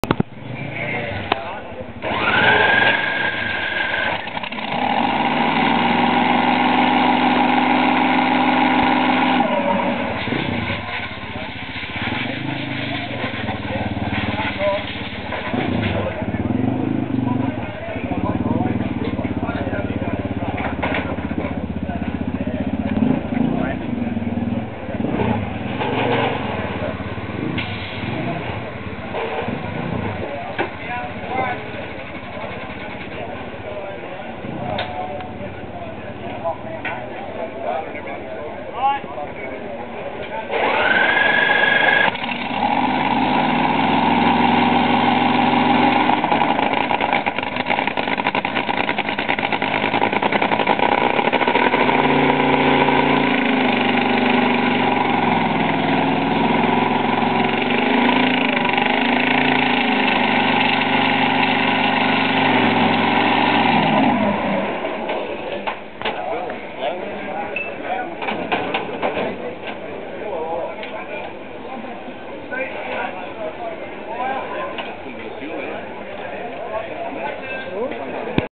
Top Fuel Dragster
A brilliant day at Willowbank Raceway for Drag Car Races and the top fuel pits are a great visit. With about 8,000 horse power this top fuel dragster thunders into life.